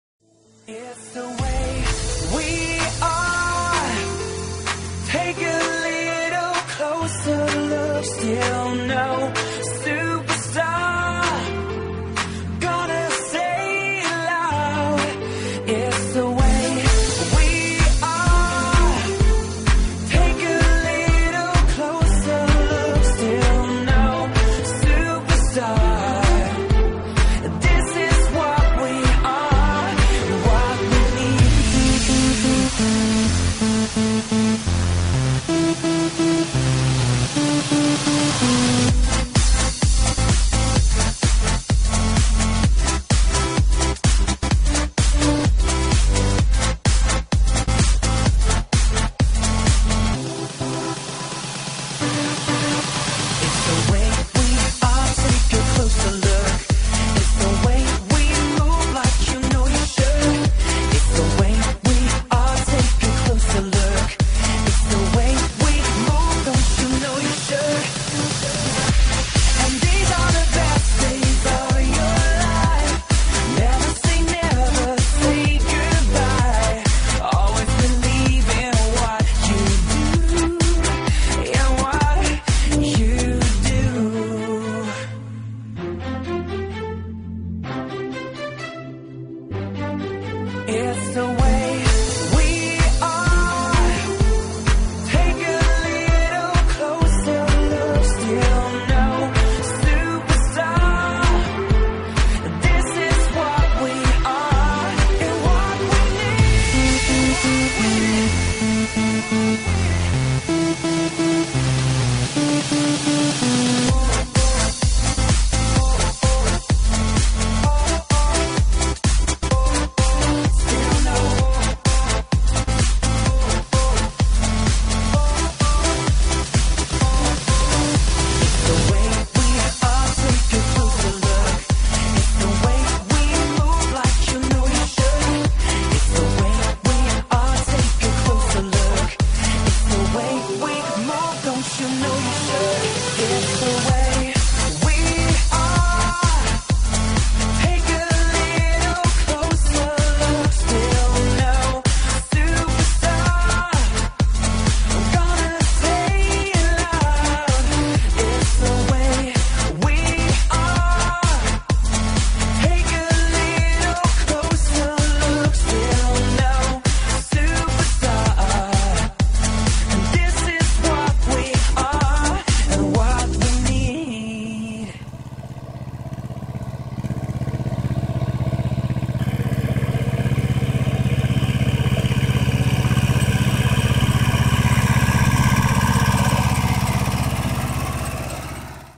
Siew pszenżyta 2016 || URSUS C330 + SIEWNIK KONNY
Witam :D Tym razem prezentuje filmik z wiosennego siewu pszenżyta.

W roli głównej:
- URSUS C330
-Siewnik konny

Ścieżka dźwiękowa, nie jest moją własnością i nie roszczę Sobie do niej praw autorskich. Film ma charakter edukacyjny.